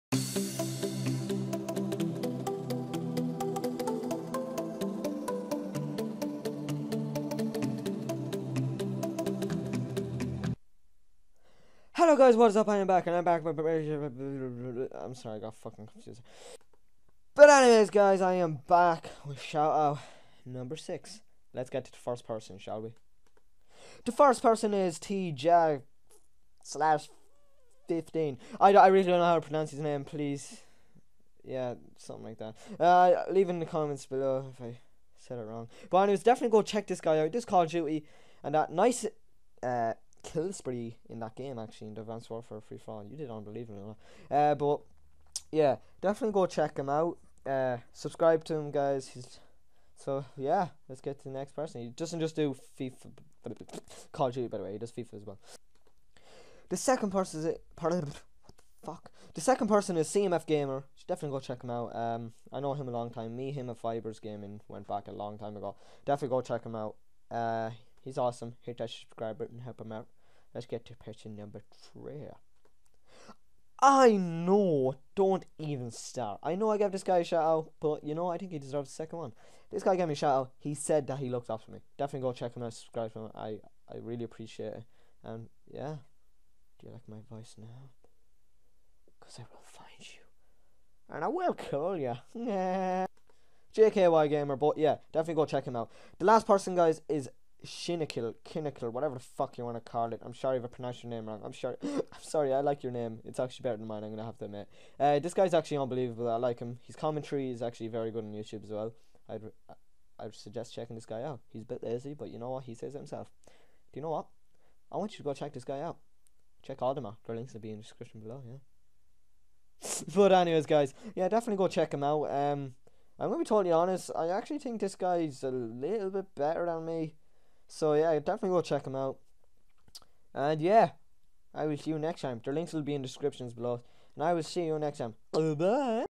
Shout out #6
Hey guys in this video I will be giving 4 youtubers a shout out.